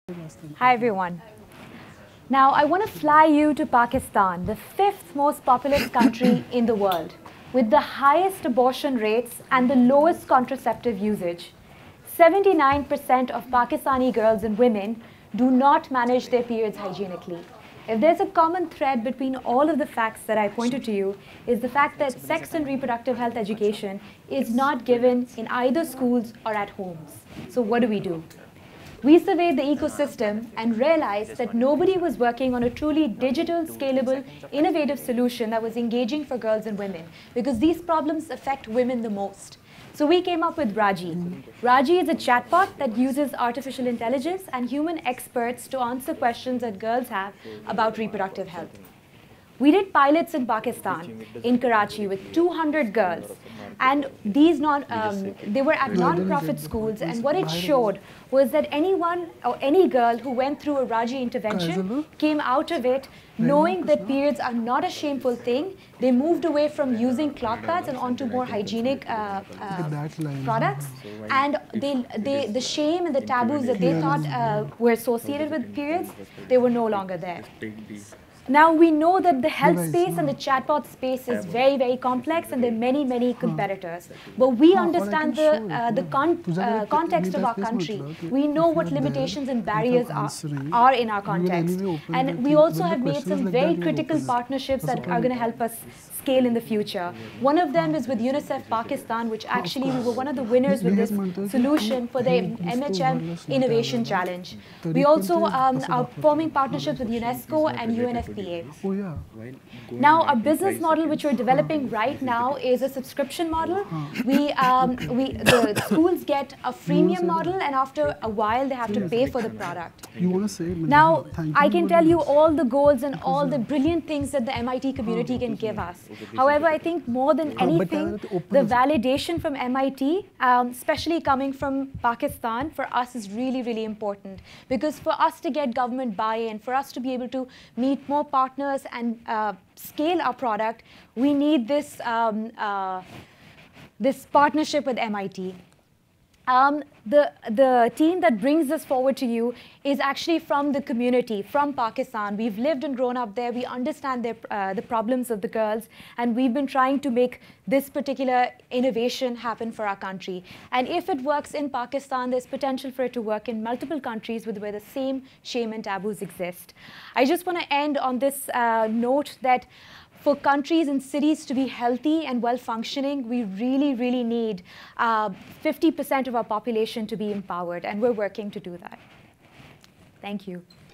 Watch Solver Saba Khalid pitch Raaji
Solver Saba Khalid pitches Raaji, an AI-powered chatbot that converses with girls on taboo health topics and connects them to professionals, products, and services, during the Healthy Cities pitch session at Solve Challenge Finals 2019.